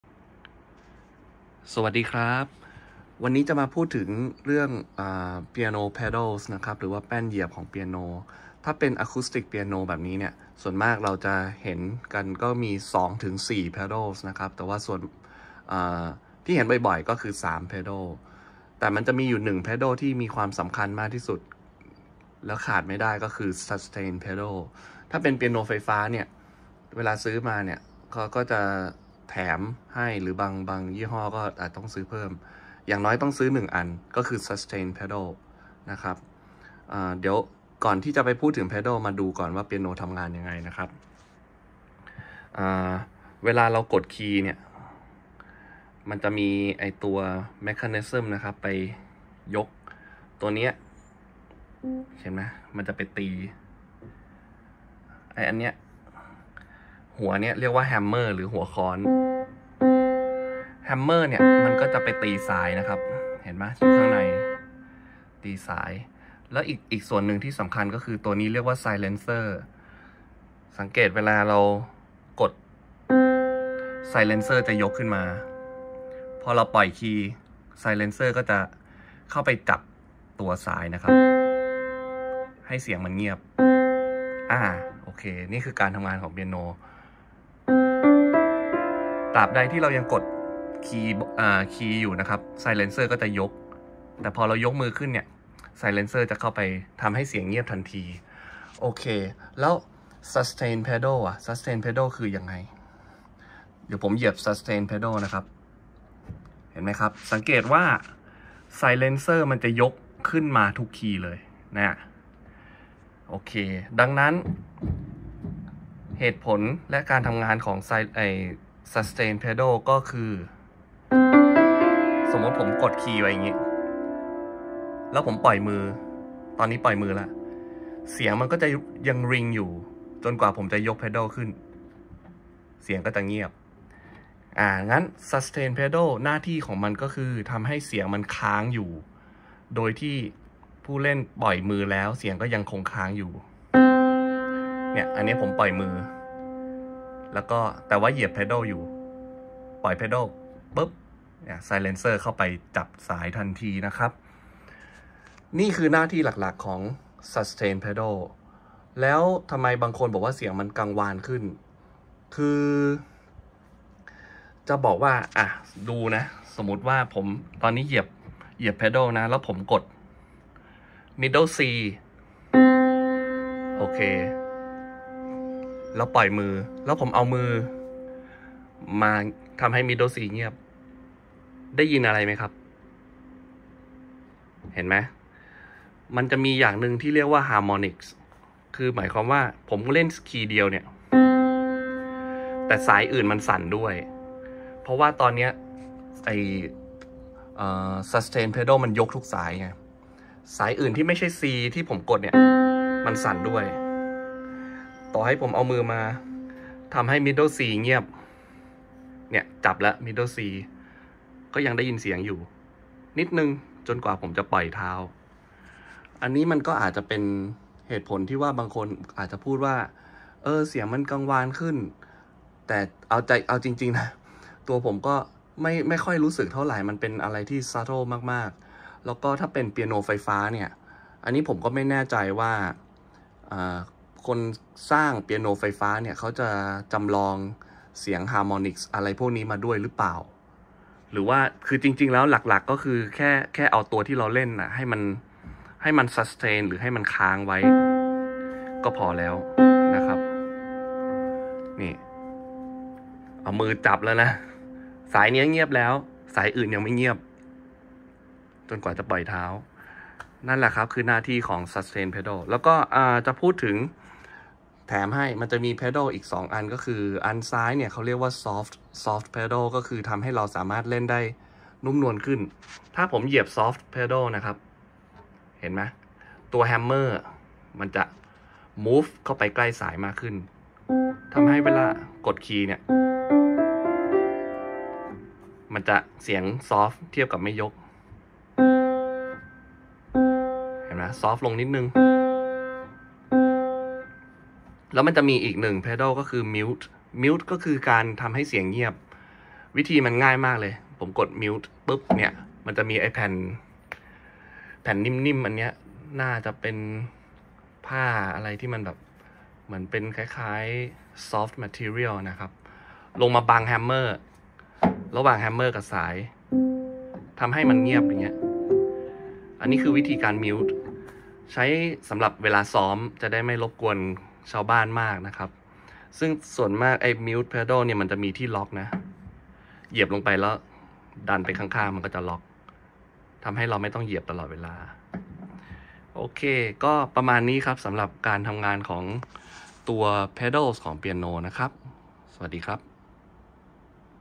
Pedals (แป้นเหยียบ) ของเปียโน มีไว้เพื่ออะไร
ว่าด้วยเรื่อง Piano Pedals (แป้นเหยียบ)
❌ ทำให้เสียงกังวาน
✅ ค้างตัวโน๊ตเอาไว้ (sustain)

** ที่ในคลิปเรียก silencer ตลอด จริงๆเรียกว่า damper นะครับ ตอนนั้นนึกไม่ออก 😂😂